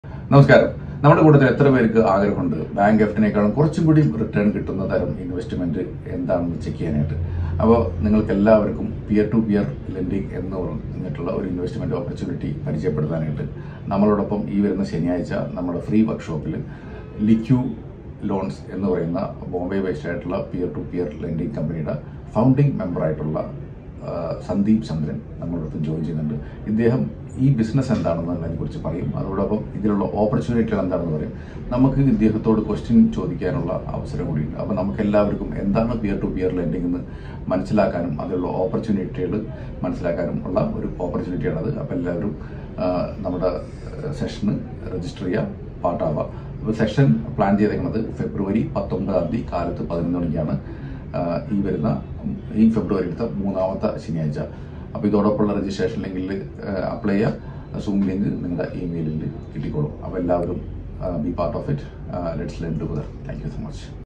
An Alternative Investment for FD - Peer to Peer Lending | Free Online Financial Session
Have you registered for our latest Free Online Financial Session yet?
Topic :An Alternative Investment for FD - Peer to Peer Lending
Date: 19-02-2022
Time: 11 am